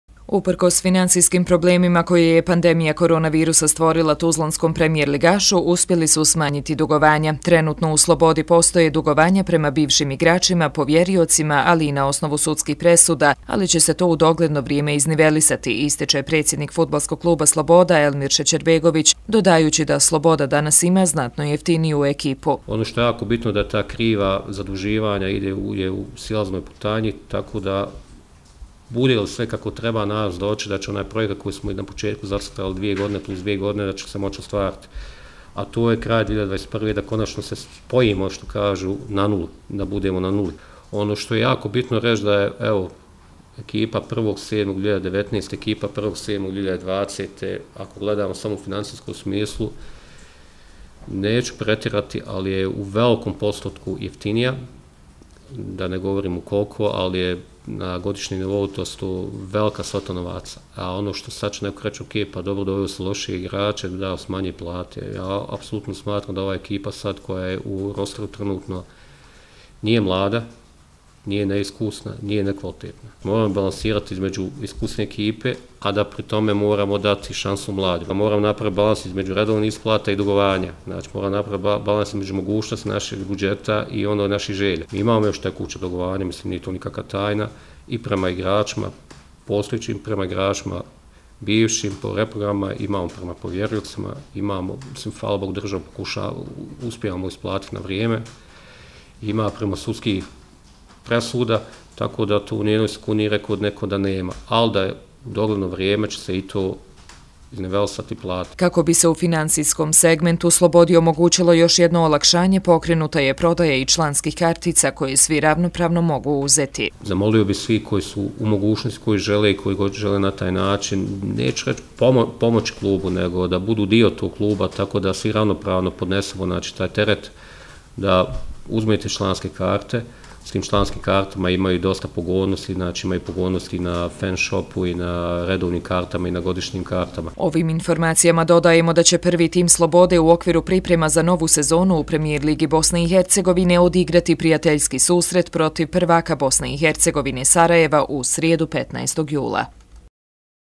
FK Sloboda: Smanjenja dugovanja, počela prodaja članskih kartica
Nešto manje od mjesec ostalo je do  početka Premijer lige BiH. Na današnjoj press konferenciji Fudbalskog  kluba Sloboda iz Tuzle istaknuto je da imaju ambicije za uspješno  djelovanje u narednom periodu, te da su smanjili dugovanja.